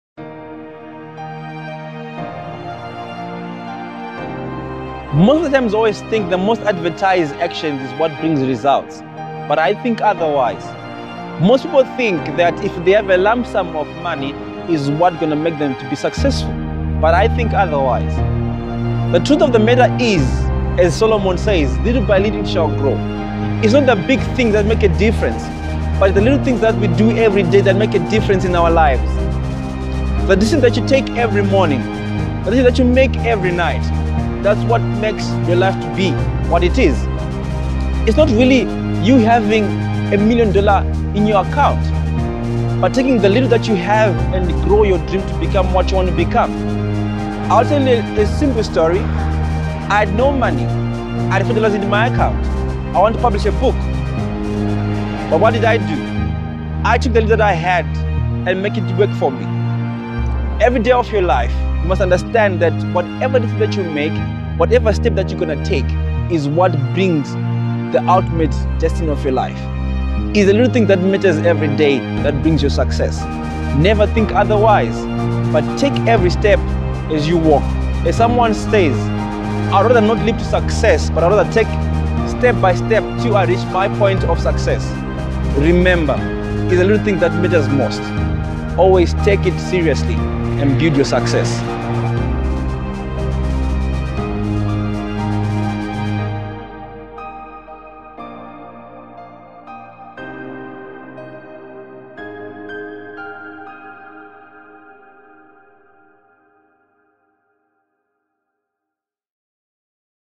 Little by little it shall grow
Inspired Life Nuggets  week 3

" each decision you take no matter how small it is important  to the  ultimate goal in your life" - Frank Nyamundero

It's never about getting a big break but doing small things great. As each small component contributes to the greater goal. Little by little it shall grow.